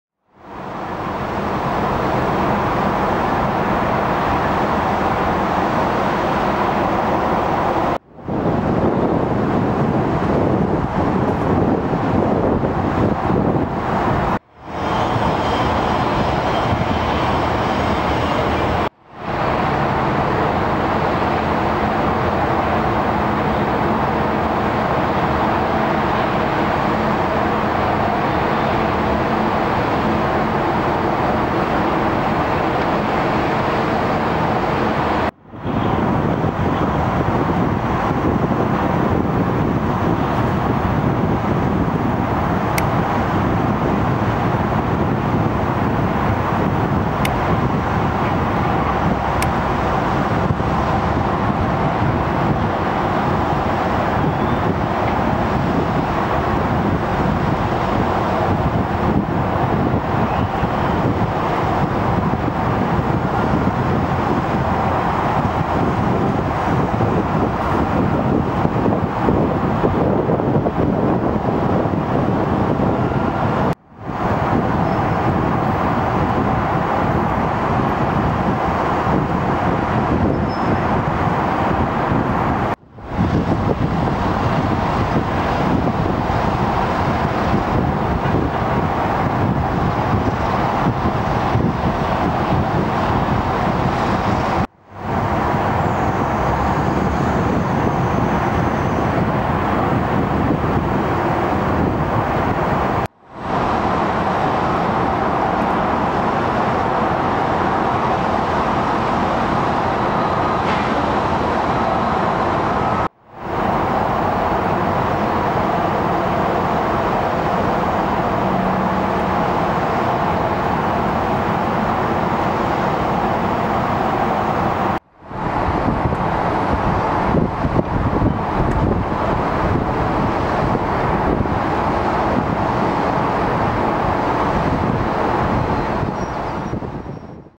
Launch of Zidell's 'Dr. Bonnie W. Ramsey'
Zidell Marine Corp. launches the 'Dr. Bonnie W. Ramsey' oil barge on July 16, 2012 into the Willamette River in Portland, Oregon.